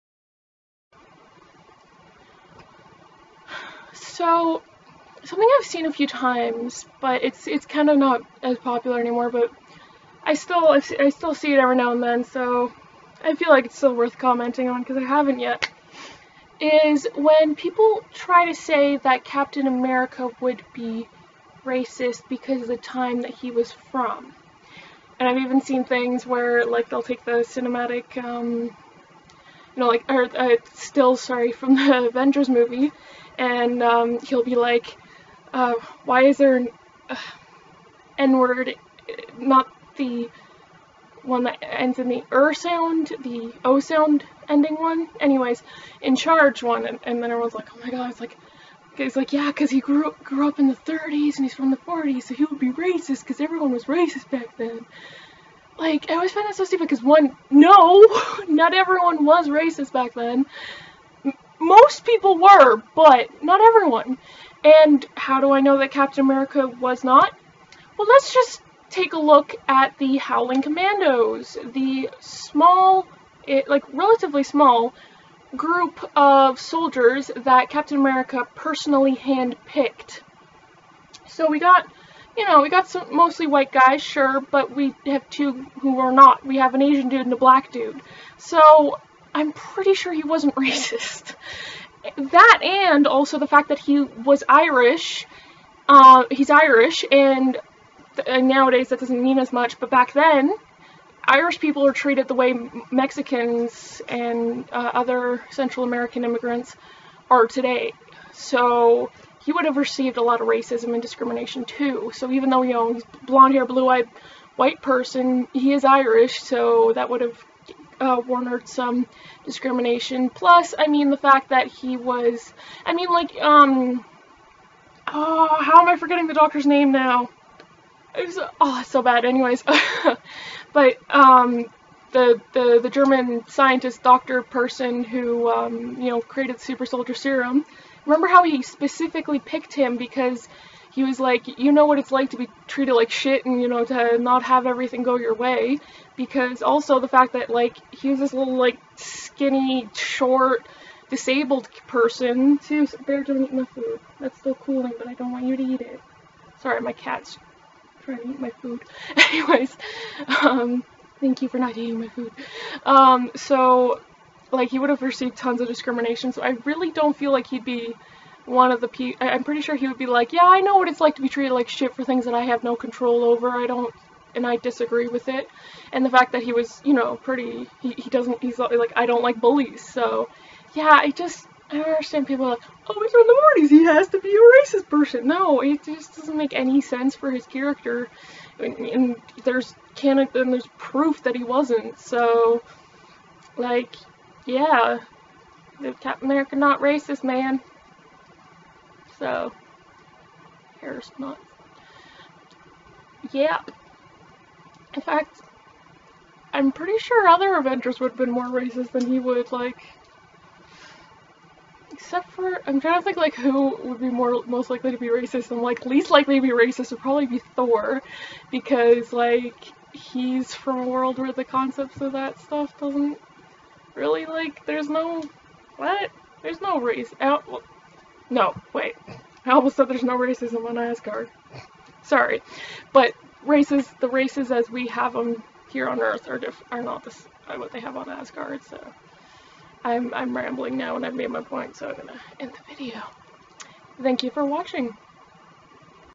Why Do People Think Captain America Would be Racist?
Like I know he's from the 40s, but he's proven not to be, and he has little reason to be. so yea. What the fuck.